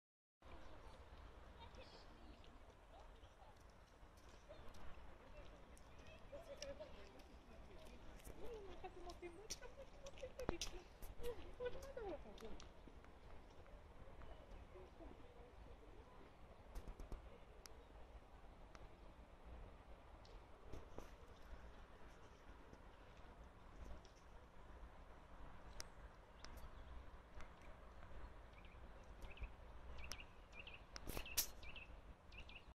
Sony Xperia Z1 Compact Video Sample II. - Mobilissimo.ro
PREȚ TELEFON ►
REVIEW ► curând
UNBOXING ►
MOSTRE FOTO ►
PE FORUM ►

Clip video realizat cu Sony Xperia Z1 Compact pentru recenzia telefonului de pe Mobilissimo.ro!

PREȚURI ACTUALIZATE:
► Marketonline:
► eMAG:
► evoMAG:
► Okazii:
► Altex: